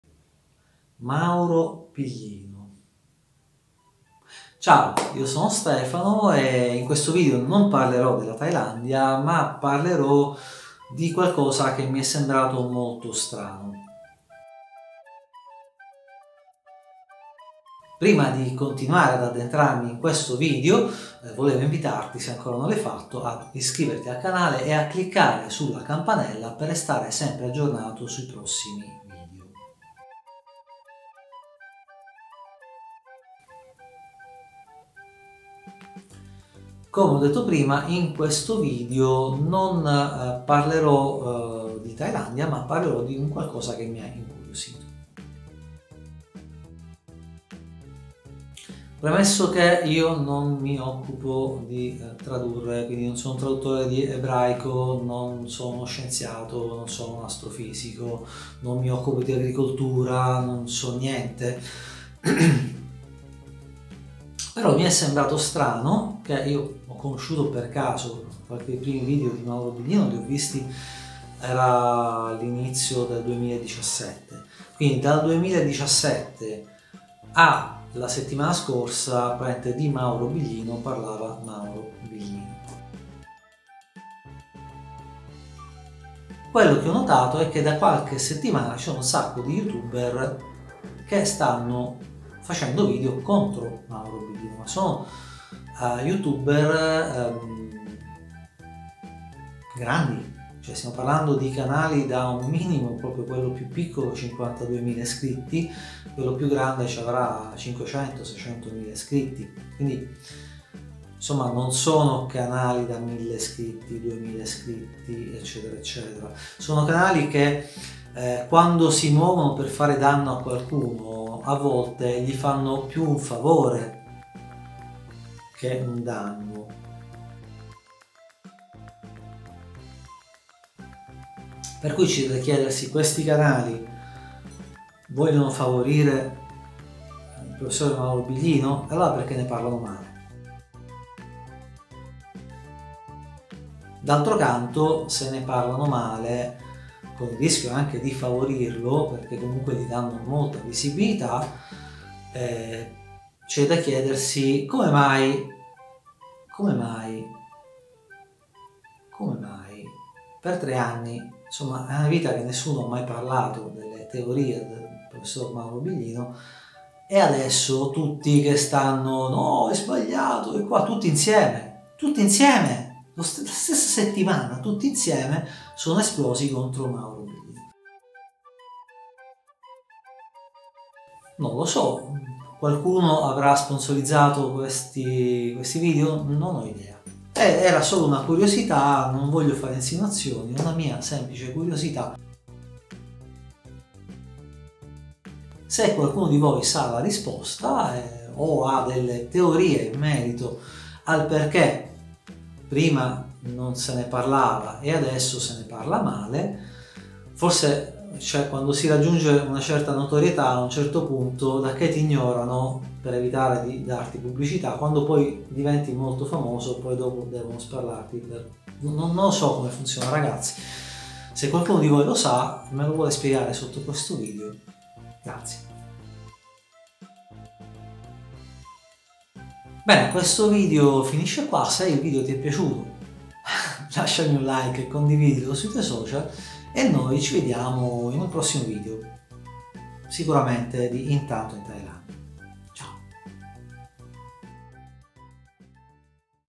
Tutti contro Mauro Biglino?
N.B. - In regalo 10 $ se ti iscrivi a Coinbase da questo

Qualcosa di strano sta succedendo su Youtube Italia... diversi youtubers, contemporaneamente, hanno pubblicato video contro Mauro Biglino... perchè?